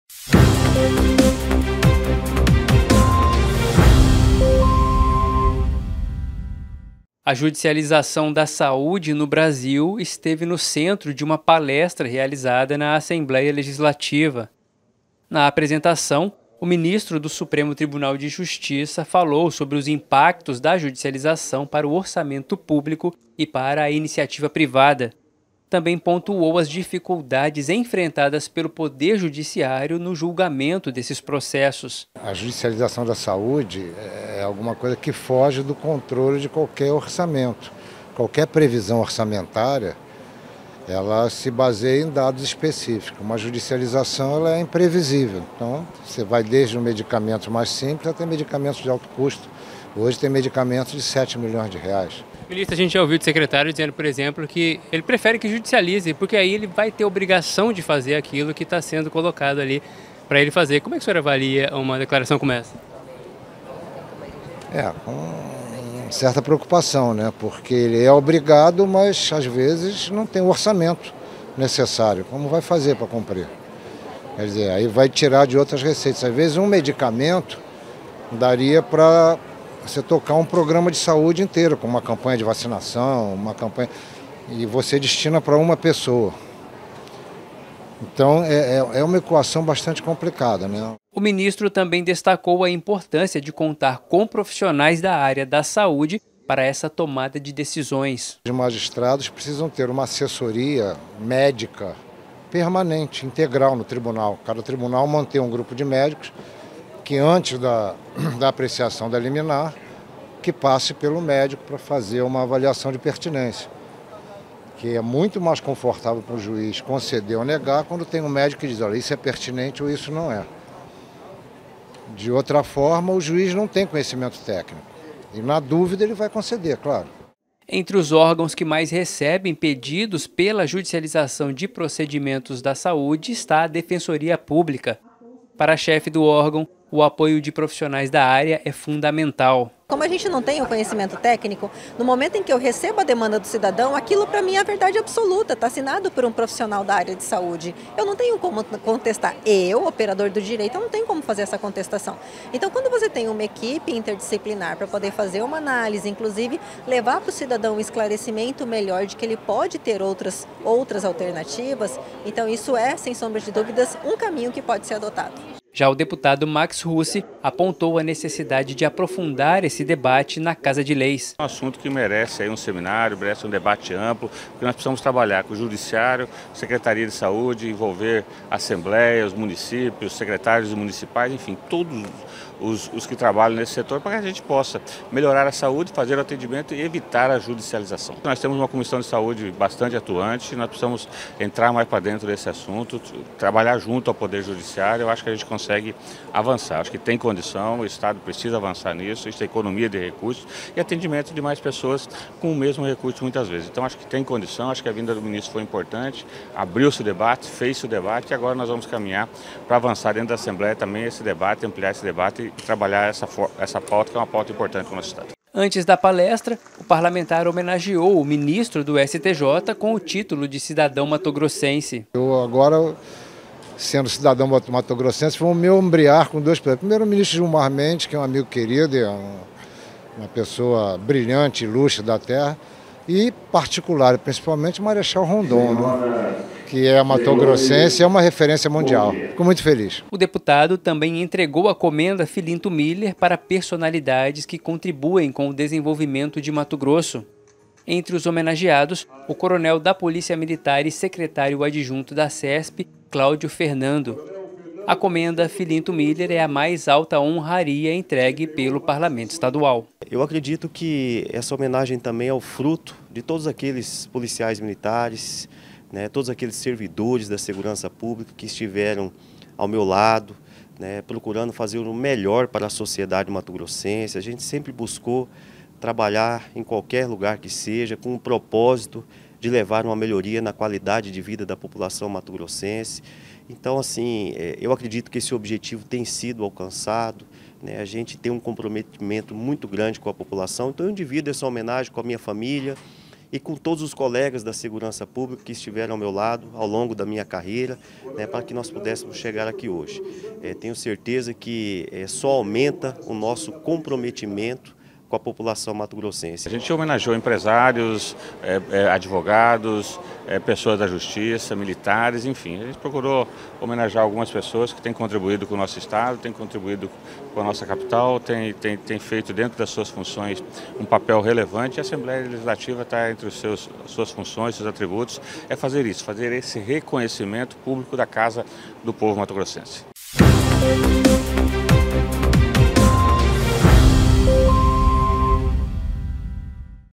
Judicialização da Saúde no Brasil
A judicialização da Saúde no Brasil esteve no centro de uma palestra realizada na Assembleia Legislativa. Na apresentação, o ministro do Supremo Tribunal de Justiça, Antonio Saldanha Palheiro, falou sobre os impactos da judicialização para o orçamento público e para a iniciativa privada. Também pontuou as dificuldades enfrentadas pelo Poder Judiciário no julgamento desses processos. Antes da palestra, o deputado Max Russi homenageou o ministro do STJ com o Título de Cidadão Mato-grossense. Também entregou a Comenda Filinto Muller para personalidades que contribuem com o desenvolvimento de Mato Grosso.